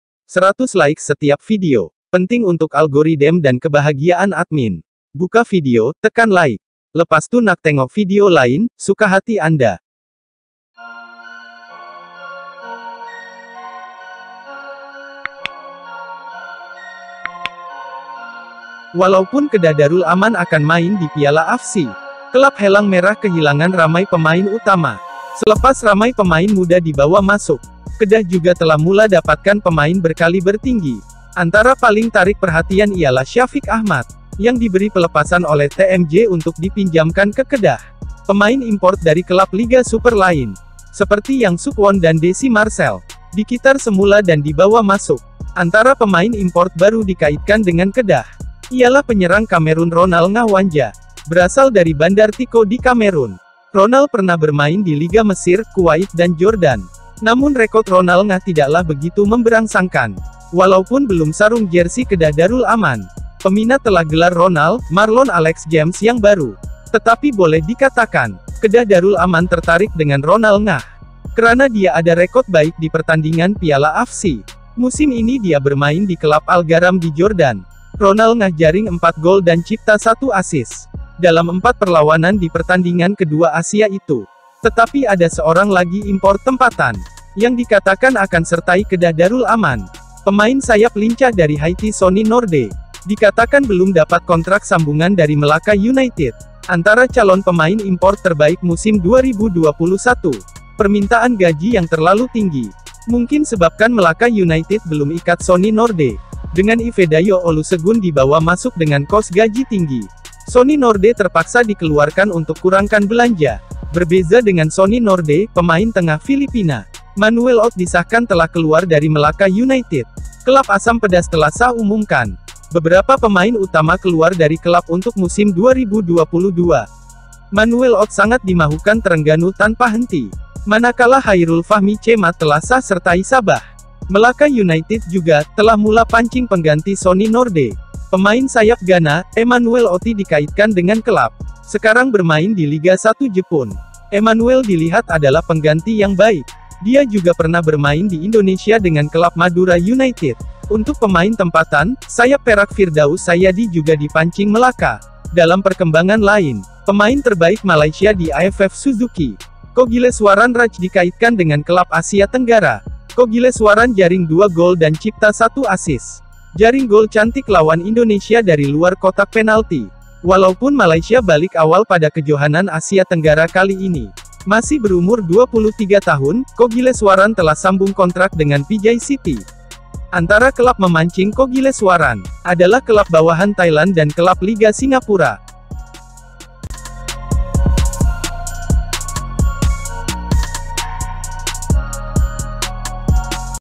Marlon Alex James Baru? Situasi Melaka United | Palatao Bola
Palatao Bola.

1. Ronald Ngah, Sony Norde dan Syafiq Ahmad gandingan baru Kedah 2022?

2. Situasi Sony Norde dan Melaka United.